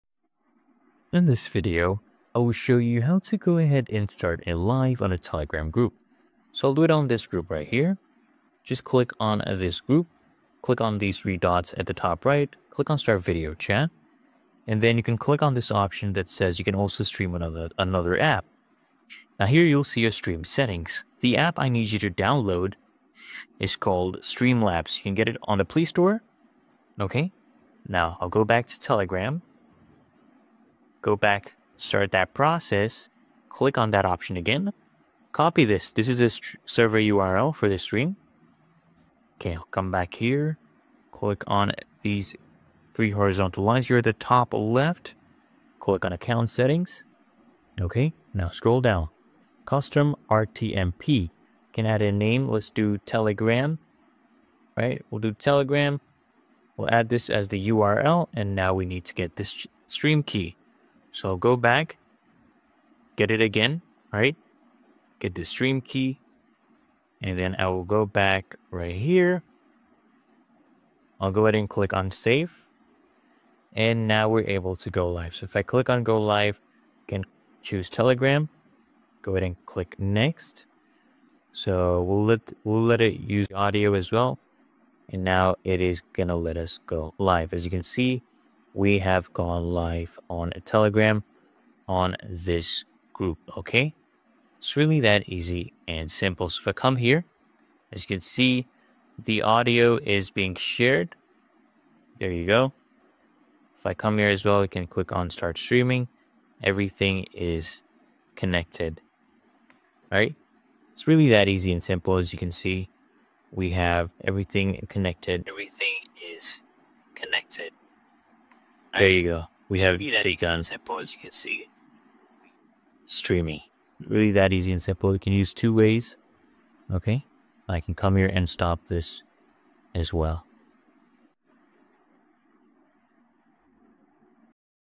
How to Start a Live Stream on a Telegram Group (Step by Step)
Want to go live on Telegram? This step-by-step guide shows you how to start a live stream in a Telegram group, manage participants, enable audio/video settings, and engage with your audience in real time. Perfect for community chats, events, and online discussions.